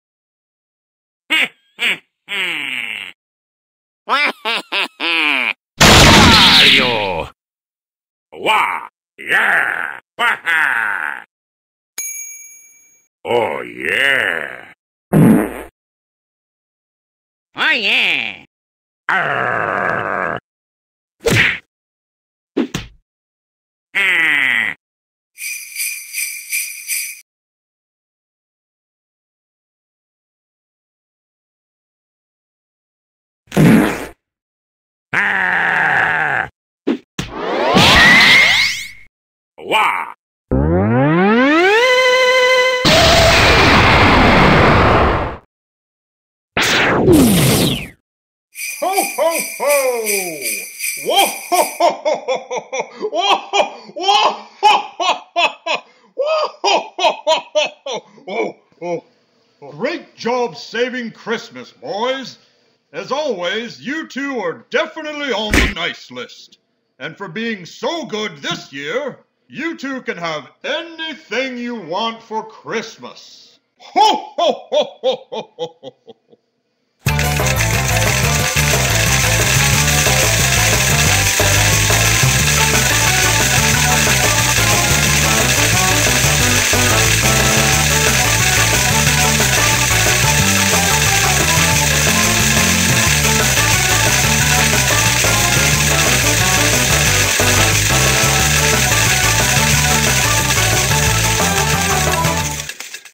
Wario and Waluigi Save Christmas
This glorious video is my Christmas special this year, and in this magnificent video, the great Wario and Waluigi save Christmas from the evil Dr. Wily. Enjoy and Merry Christmas!